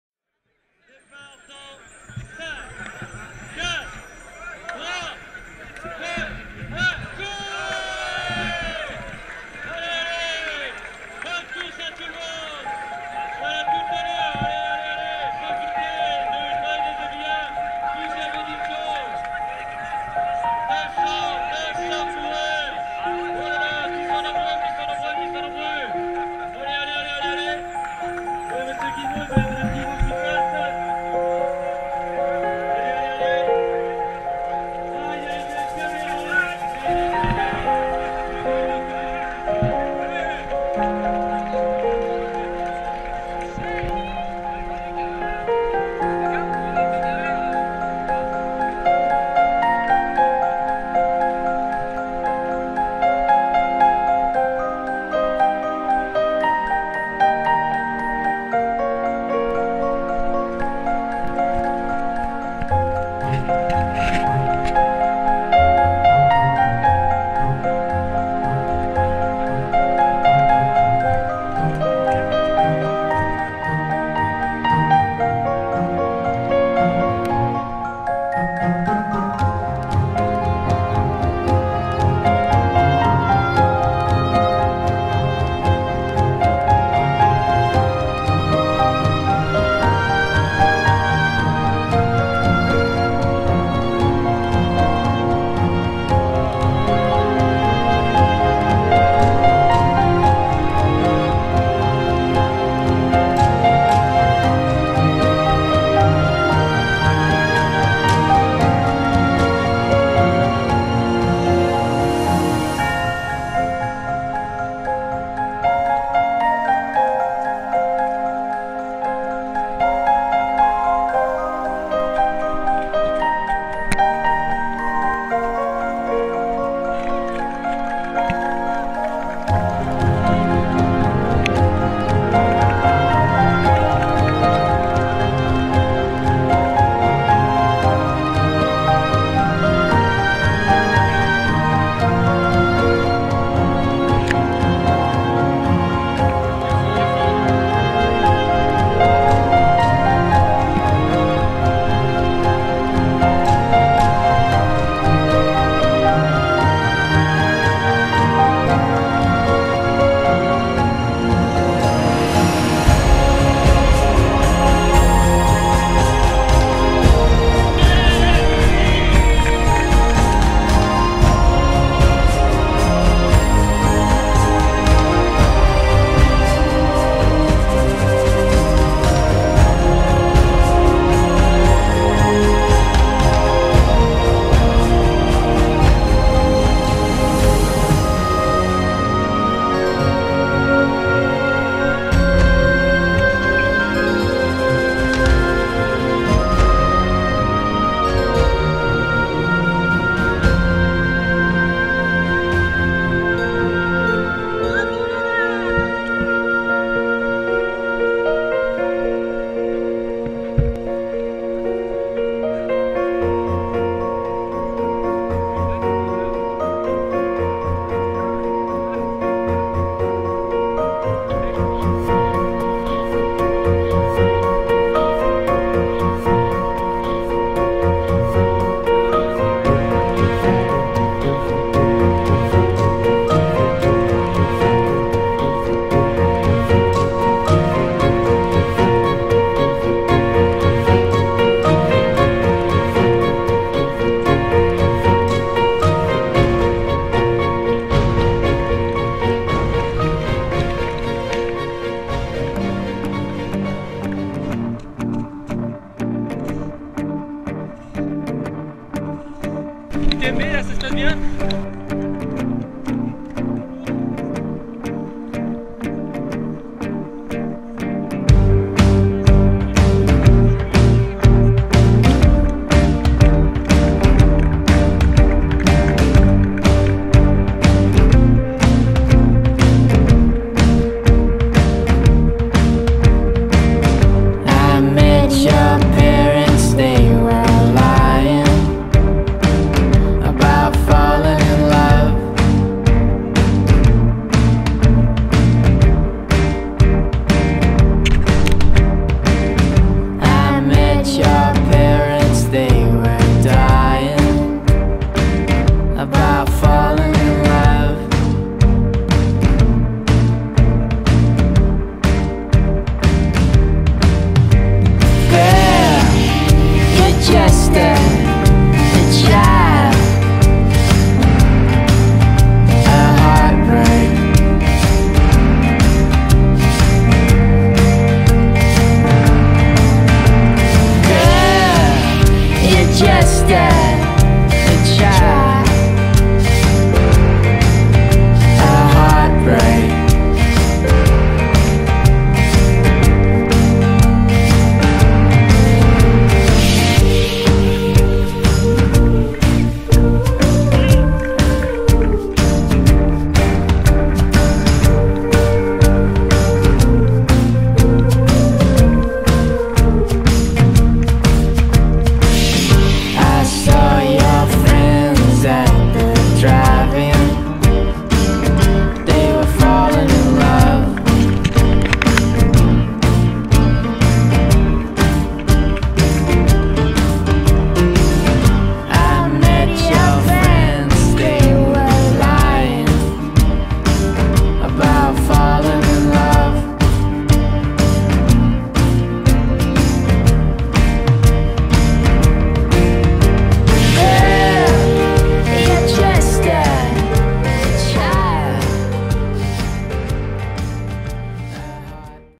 Trail Ebihens 2015
Trail de 20,3 km entre Saint-Jacut de la Mer et les Ebihens